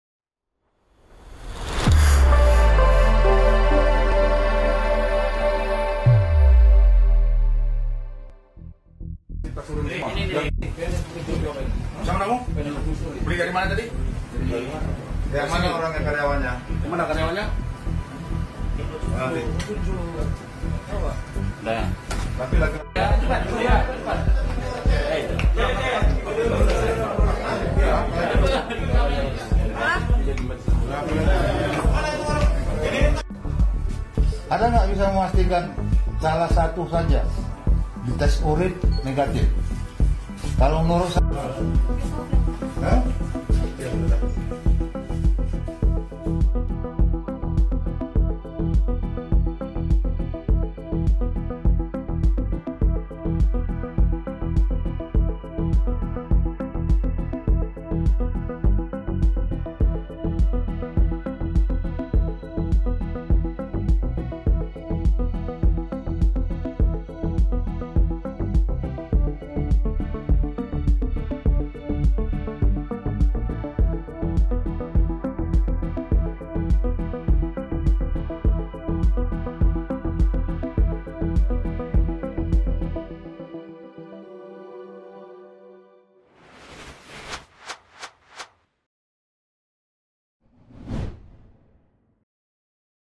SEKDA KABUPATEN NIAS UTARA DI TANGKAP TERKAIT NARKOBA
Sekretaris Daerah (Sekda) Kabupaten Nias Utara, Yafeti Nazara ditangkap terkait narkoba.

Yafeti ditangkap di Jalan Adam Malik, Medan, sekitar pukul 02.30 WIB, Minggu (13/6/2021) bersama tiga wanita di tempat karaoke

"Iya benar. Iya Yafeti Nazara, Sekretaris Daerah (Sekda) Kabupaten Nias Utara," kata Amizaro Waruwu saat dimintai konfirmasi, Senin (14/6/2021).

Pemerintah Kabupaten Nias Utara buka suara terkait Yafeti Nazara, yang ditangkap karena narkoba di Medan. Yafeti disebut akan dicopot dari jabatannya.

Via : jayalah.negriku